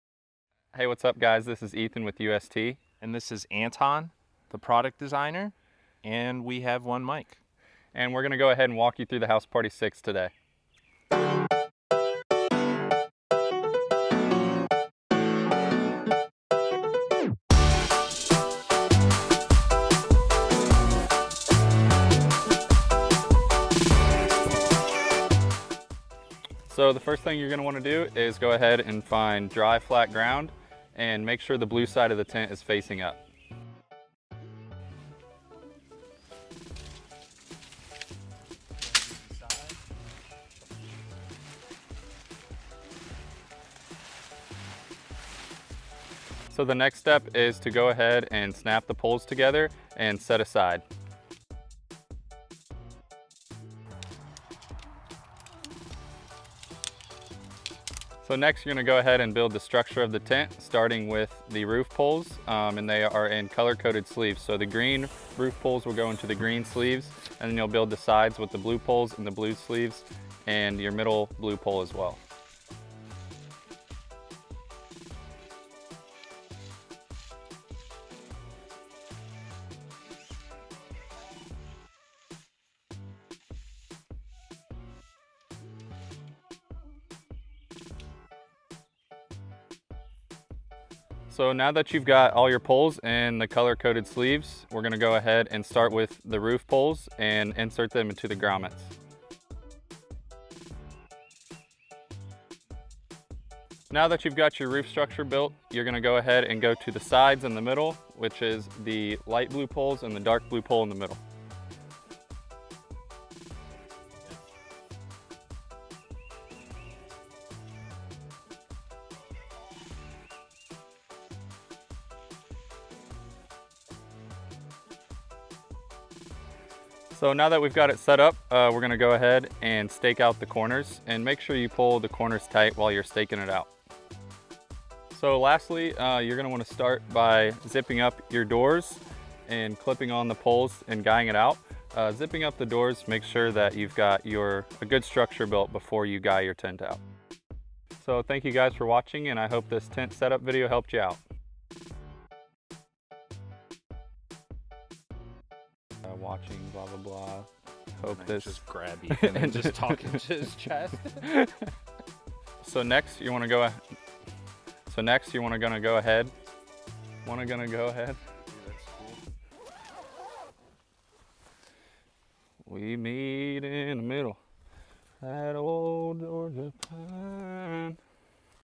how to | setting up our house party 6p tent
In this video we walk you through the set up of our 6 person tent.